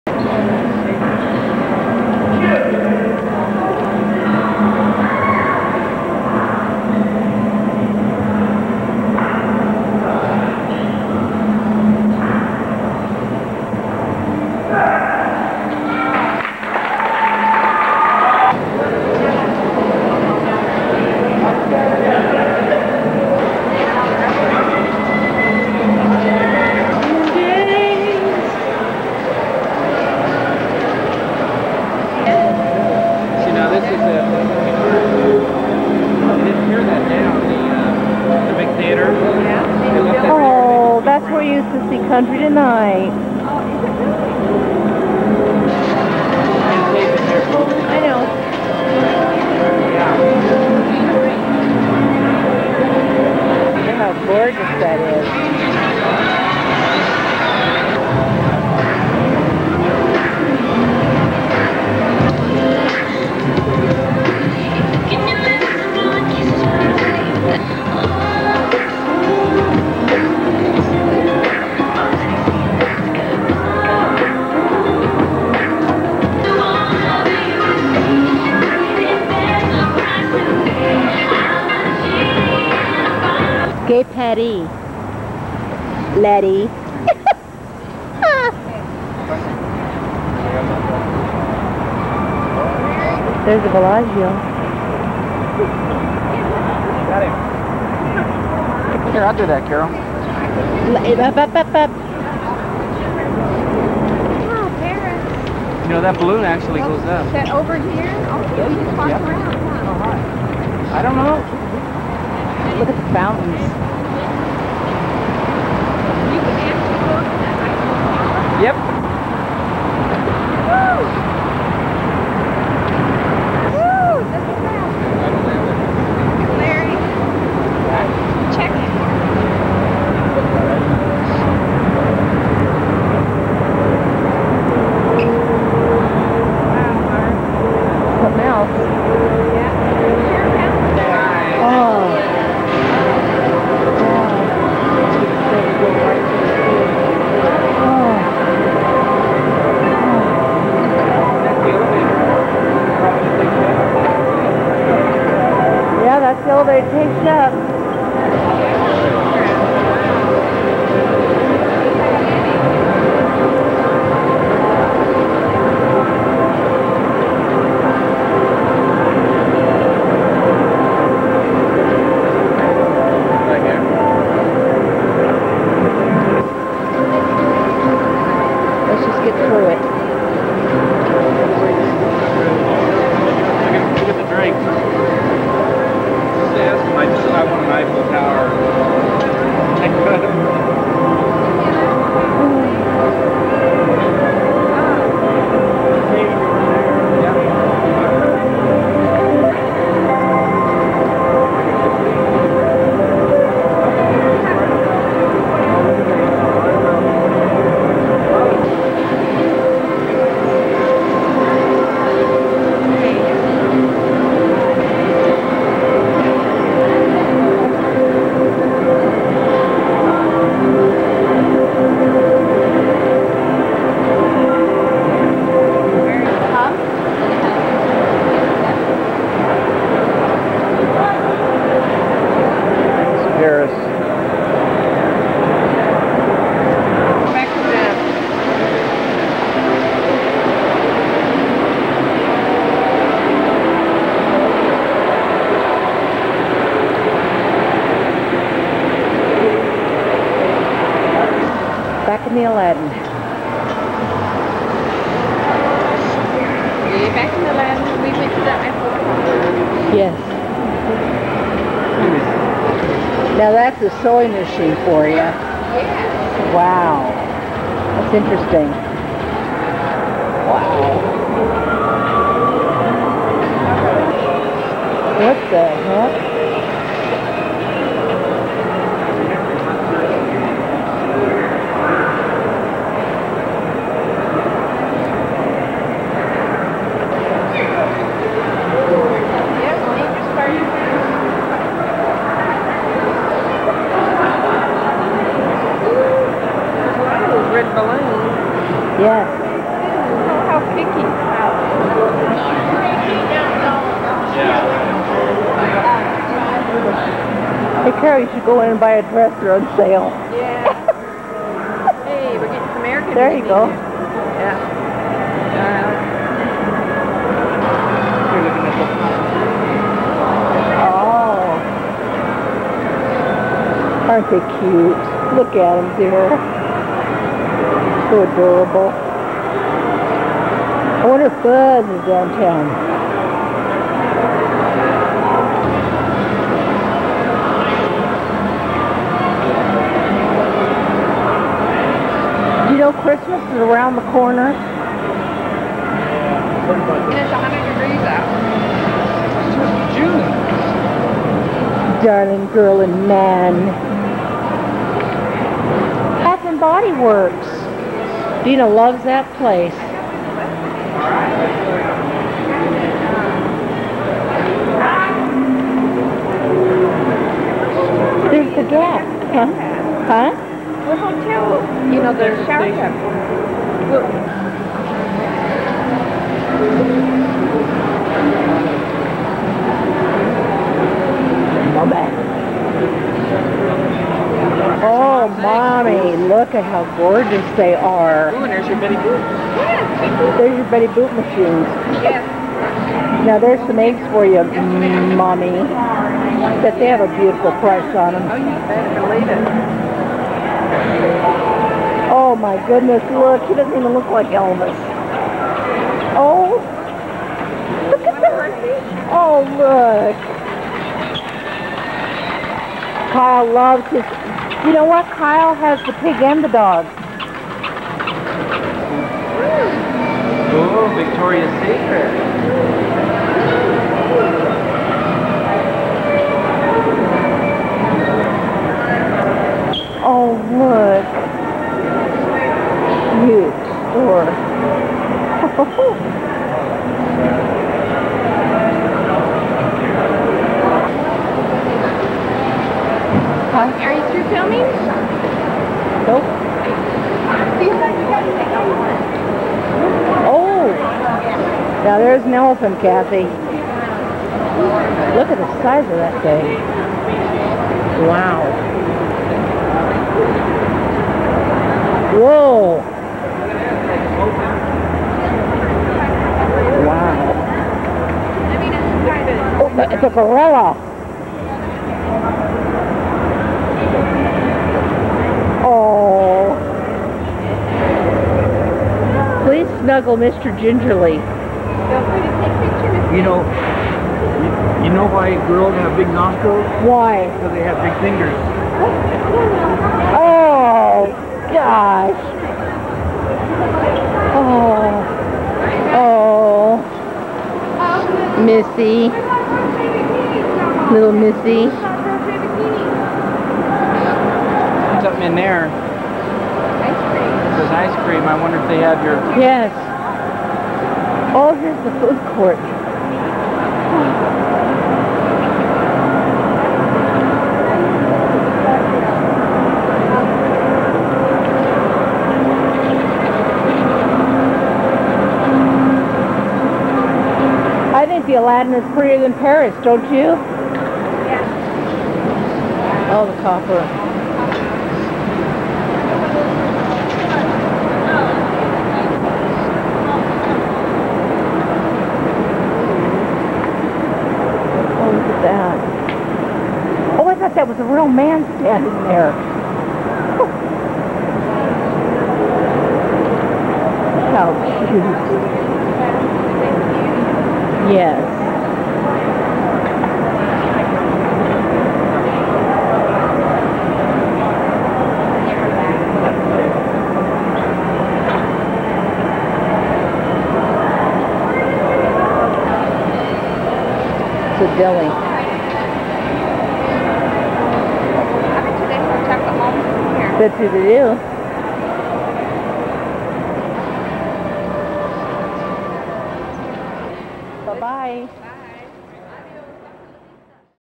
Strolling Around Vegas
Enjoying Las Vegas with friends in the 80's.